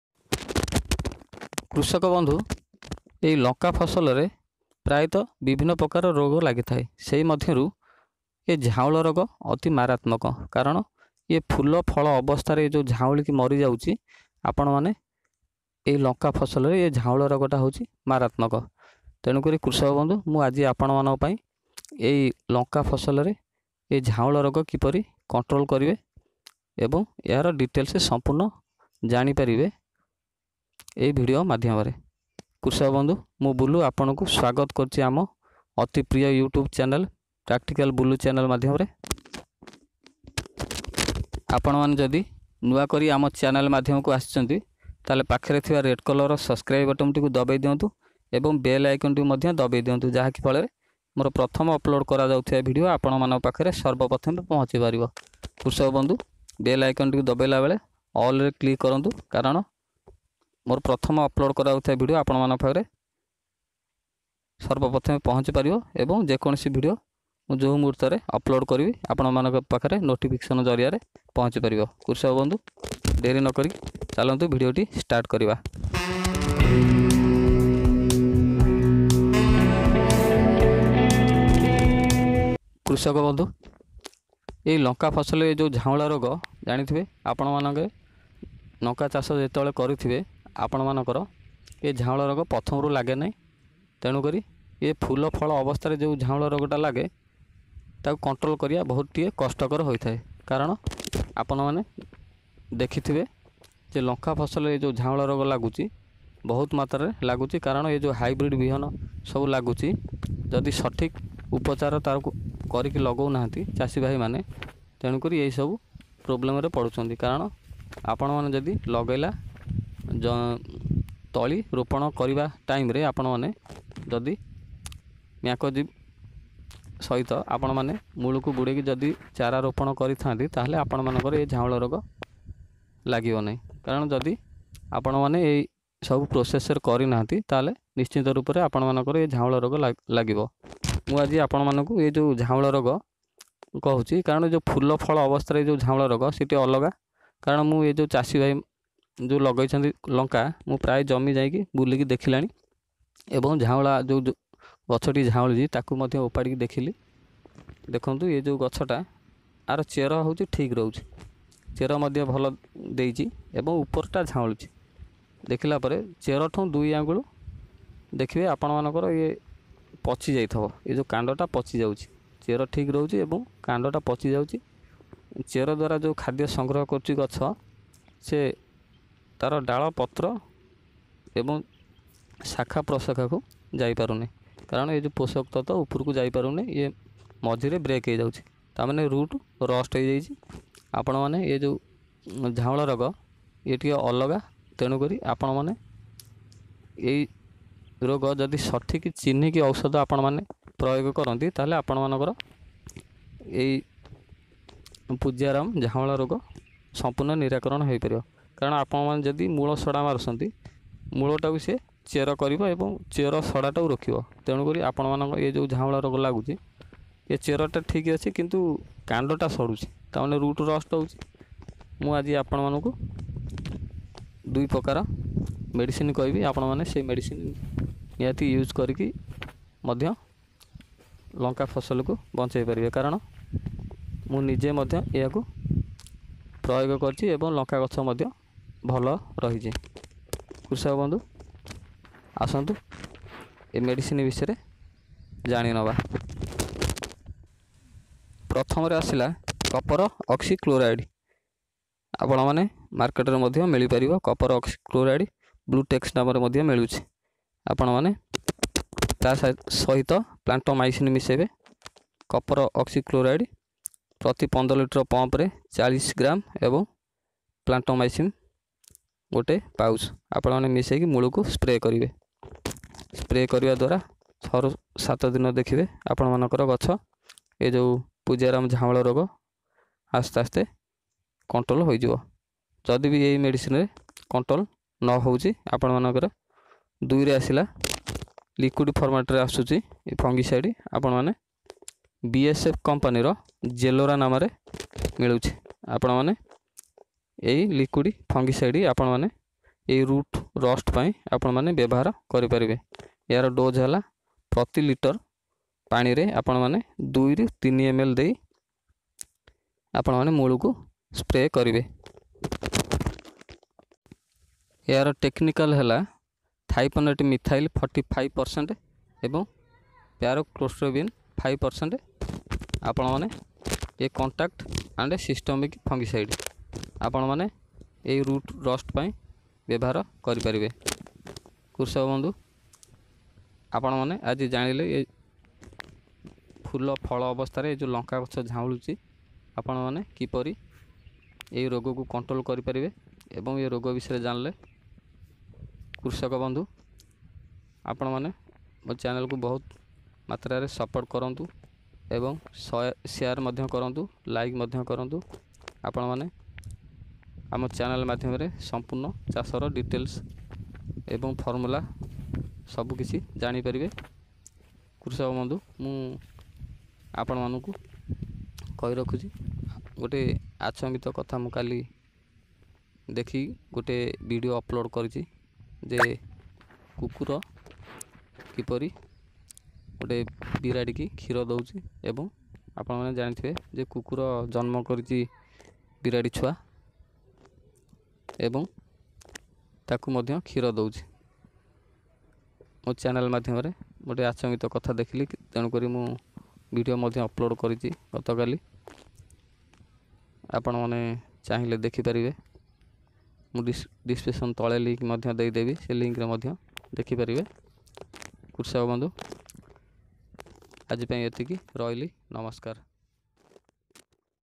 ଲଙ୍କା ଫସଲରେ ଝାଉଁଳା ରୋଗ ନିୟନ୍ତ୍ରଣ କିପରି କରିବେ //how to control fungos your chili farm
କୃଷକବନ୍ଧୁ ଲଙ୍କା ଗଛ ଝାଉଁଳା କିପରି ନିୟନ୍ତ୍ରଣ କରିବେ ଏହି ଭିଡ଼ିଓ ମାଧ୍ୟମରେ ଦେଖିପାରିବେ